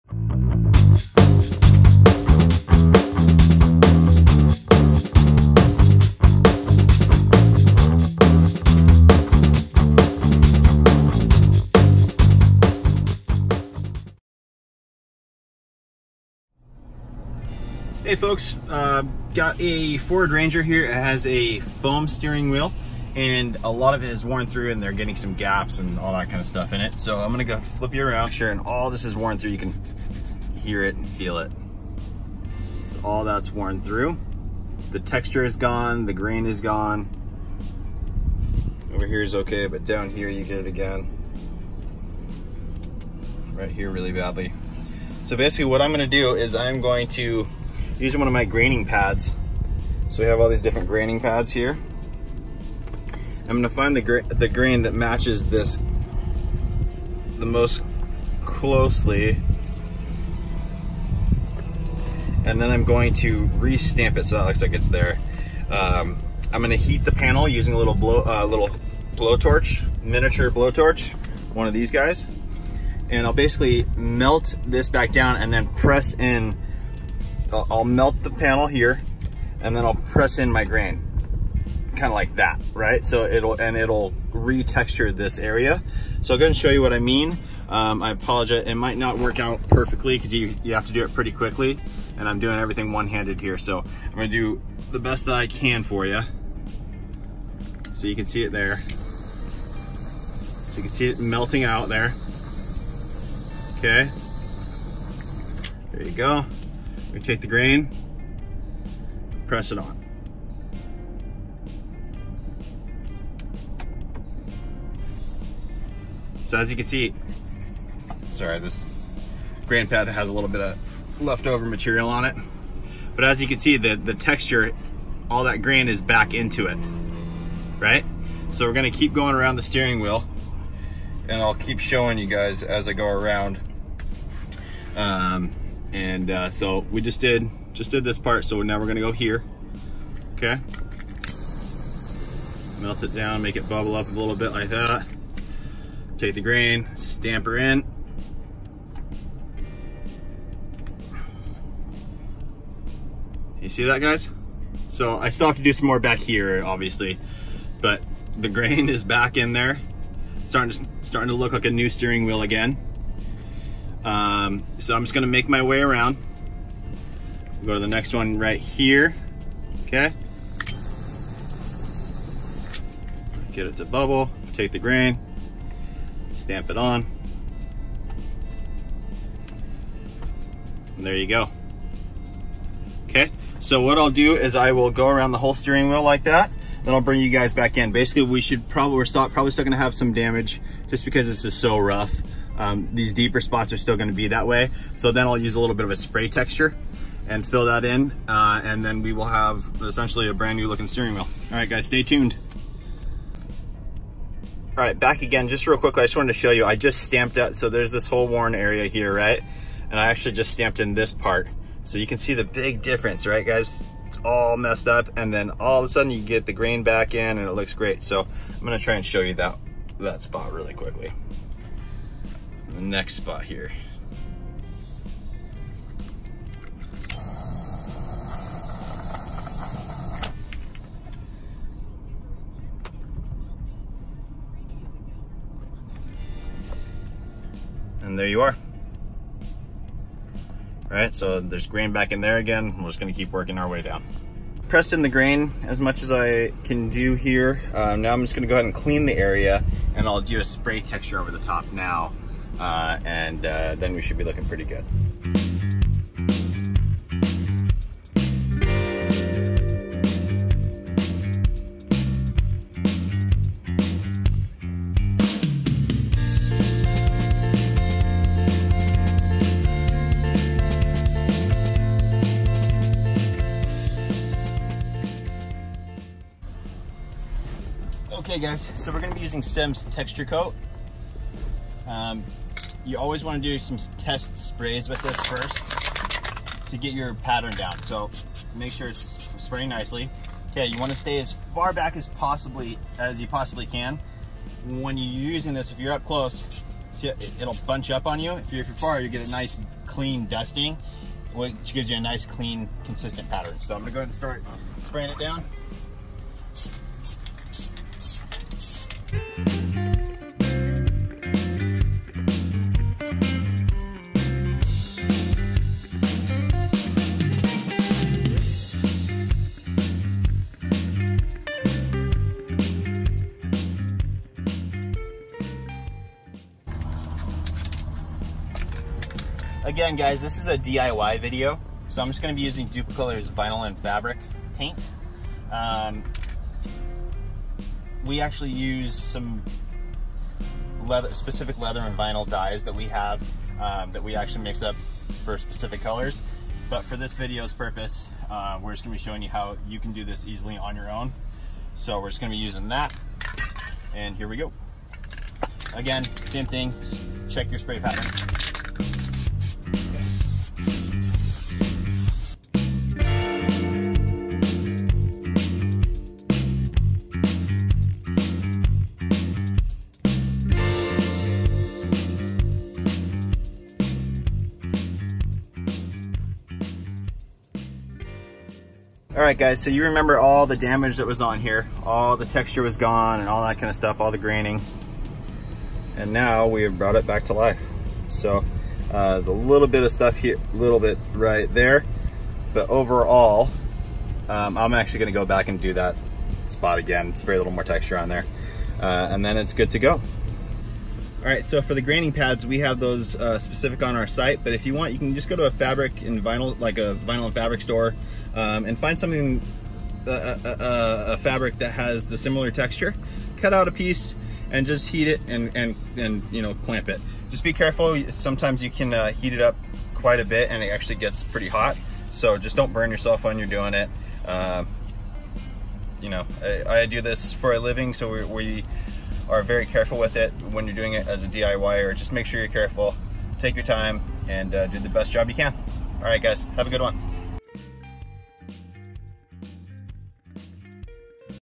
DIY Steering Wheel Repair | How to fix your steering wheel
In todays video, we provide a step by step guide on how to repair a foam based steering wheel.

Foam based steering wheels are used in vehicles ranging from Hyundai and Ford to Mercedes.  I just recently bought a 2019 Mercedes Sprinter Van that has that type of wheel.  With the steering wheel being hit constantly by the sun through the windshield, oils in your hands, constant friction from driving... Steering wheels have a tendency to break down.  We show you how you can reseal the foam and stamp in a grain to match the original texture of your wheel!

Steering wheel repair step 1: Inspect the area.  Make sure that your steering wheel is foam based before trying this repair method.  If it is not foam based.. refer to this video

Steering wheel repair step 2: Using a micro torch or heat gun, heat the a small portion of the steering wheel. A small enough are that the graining pad will cover.  Once the material is hot it will become soft and you can then press in the grain which also reconstitutes the wheel.

Steering wheel repair step 3: Continue your way around the wheel until all of the damaged area is re sealed and textured

Steering wheel repair step 4: Now using a cleaner, wipe down the whole steering wheel to remove any oils, dirt, or other contaminants

Steering wheel repair step 4: Cover all areas to prevent overspray and the light spray even coats to the steering wheel to restore the color.  Light coats are preferred so that you do not get runs!